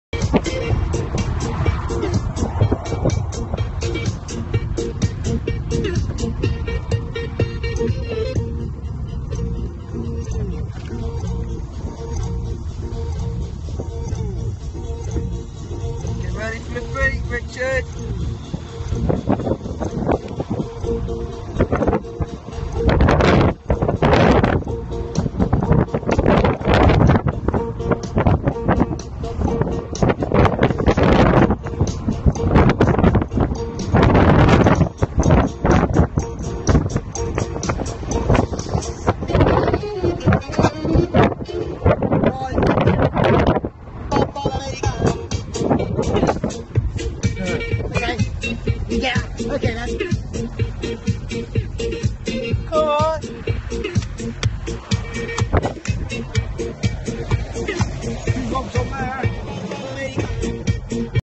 Bouns Clip Dashcam Driving
Our friend recoreded us singing and dancing